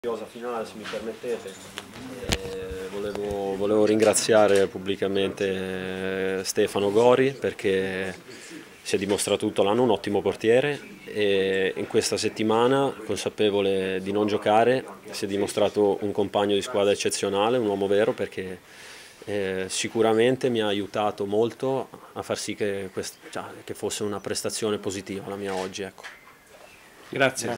10/04/2023 POST PARTITA IN ZONA INTERVISTE DI JACOPO FURLAN
Riprese 7oz. Photo Agency - Stefano Vissicchio - Riproduzione vietata
Intervista Alessio Torzuoli